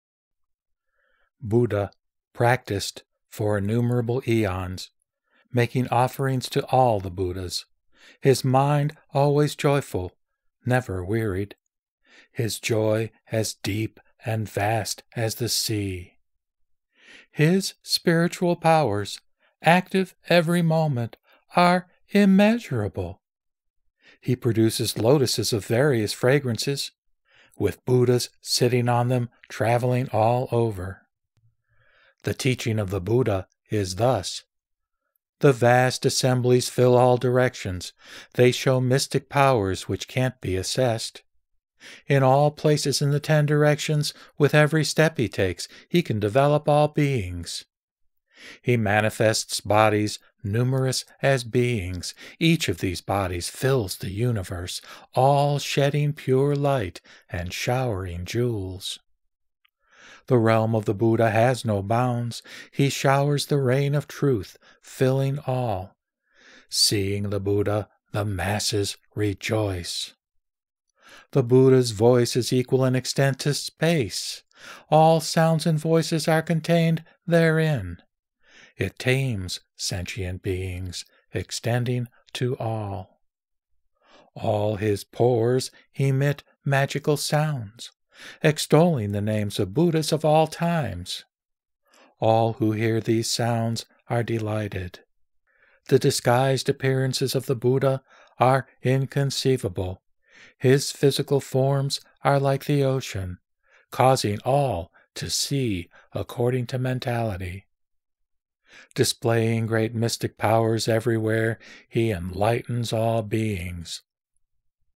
The Buddha's Qualities Pt. 37
Verses spoken by the footstep-following spirit Precious Symbol, in the chapter called The Wonderful Adornments of the Leaders of the World, of The Flower Ornament Scripture (Avatamsaka Sutra), translated by Thomas Cleary, Shambhala Publications, Inc.